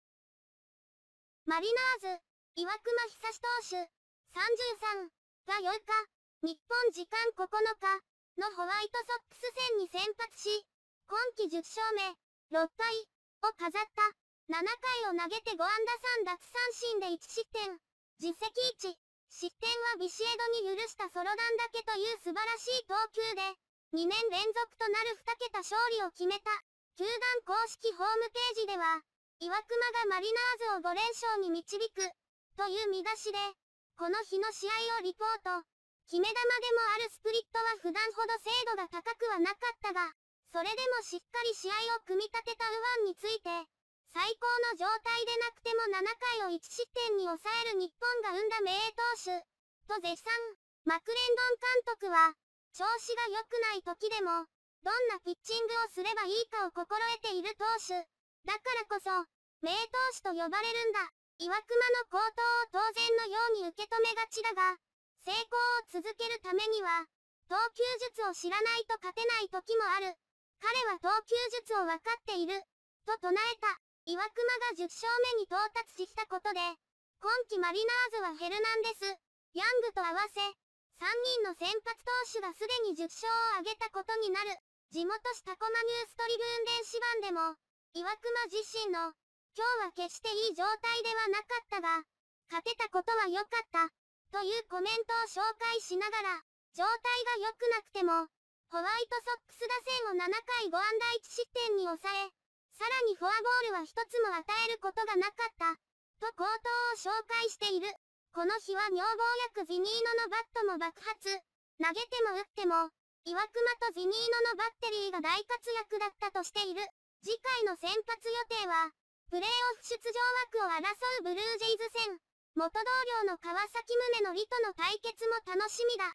「日本が生んだ名投手」 岩隈久志、絶賛される
マリナーズ岩隈久志投手（33）が8日（日本時間9日）のホワイトソックス戦に先発し、今季10勝目（6敗）を飾った。7回を投げて5安打3奪三振で1失点（自責1）。失点はビシエドに許したソロ弾だけという素晴らしい投球で、2年連続となる2桁勝利を決めた。

決め球でもあるスプリットは普段ほど精度が高くはなかったが、それでもしっかり試合を組み立てた右腕について「最高の状態でなくても7回を1失点に抑える日本が生んだ名投手」と絶賛。

　マクレンドン監督は「調子がよくない時でも、どんなピッチングをすればいいかを心得ている投手。だからこそ、名投手と呼ばれるんだ。岩隈の好投を当然のように受け止めがちだが、成功を続けるためには、投球術を知らないと勝てない時もある。彼は投球術を分かっている」と称えた。

　岩隈が10勝目に到達したことで、今季マリナーズはヘルナンデス、ヤングと合わせ、3人の先発投手がすでに10勝を挙げたことになる。

　地元紙タコマ・ニュース・トリビューン電子版でも、岩隈自身の「今日は決していい状態ではなかったが、勝てたことはよかった」というコメントを紹介しながら、「状態がよくなくても、ホワイトソックス打線を7回5安打1失点に抑え、さらにフォアボールは1つも与えることがなかった」と好投を紹介している。

　この日は女房役ズィニーノのバットも爆発。投げても打っても、岩隈とズィニーノのバッテリーが大活躍だったとしている。

　次回の先発予定は、プレーオフ出場枠を争うブルージェイズ戦。元同僚の川崎宗則との対決も楽しみだ。